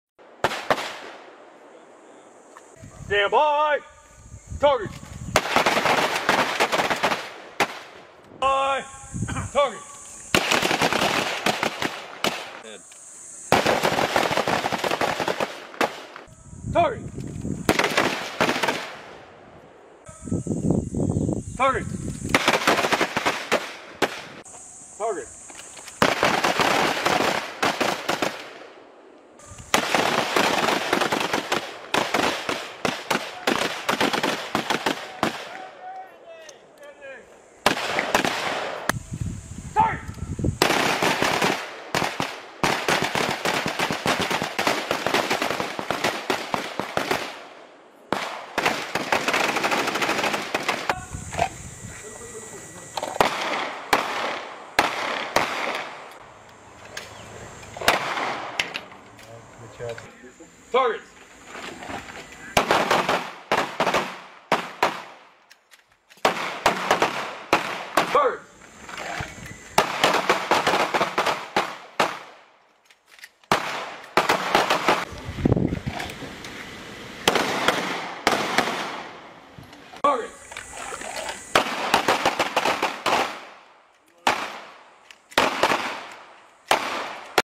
Philippine Marine Recon Battalion
Philippine Marine Recon Battalion alongside with US Marine Force Recon conduct a, live-fire range, during Balikatan 23 at Marine Base Gregorio Lim, Philippines, April 16, 2023.